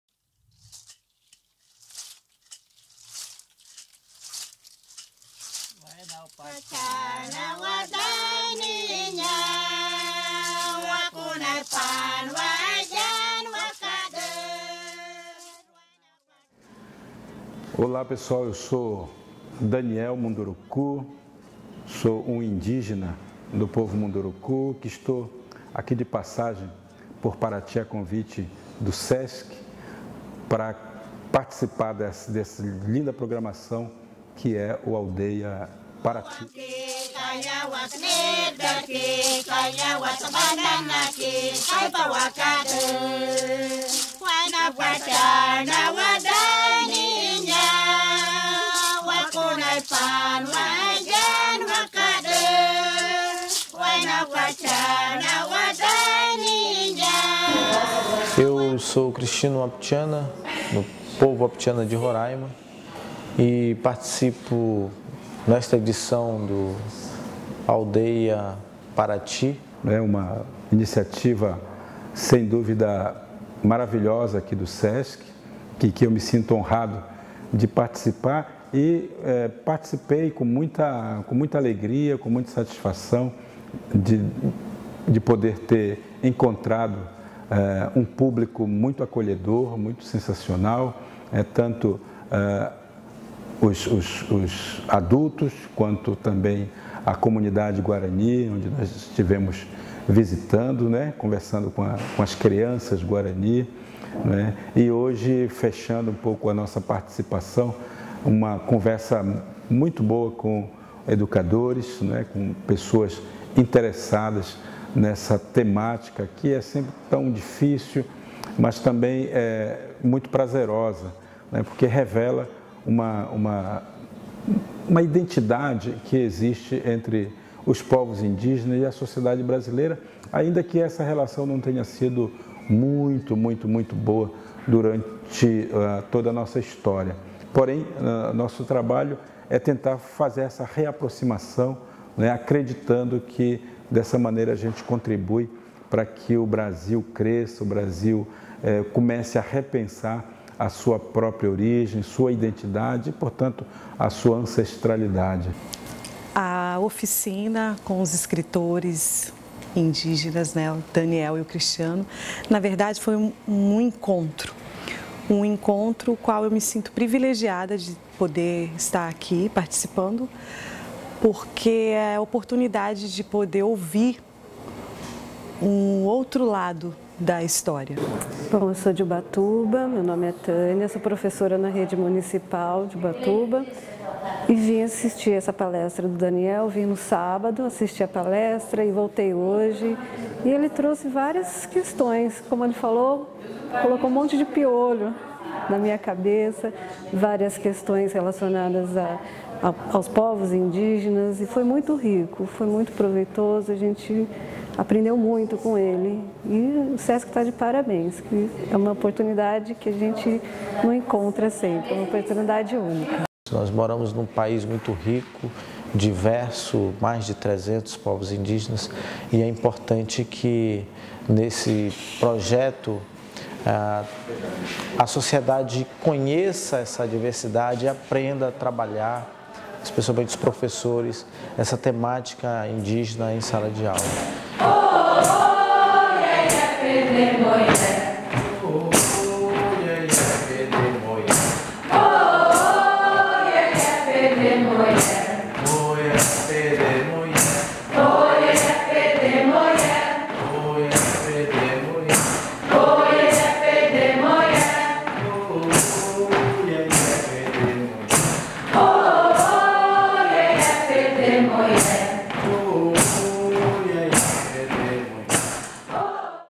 O BANQUETE DOS DEUSES
Bate Papo com os escritores indígenas Daniel Munduruku e Cristino Wapichana.

Créditos do Vídeo:

Direção: Antonio Garcia Couto
Câmeras: Antonio Garcia Couto e Marcus Prado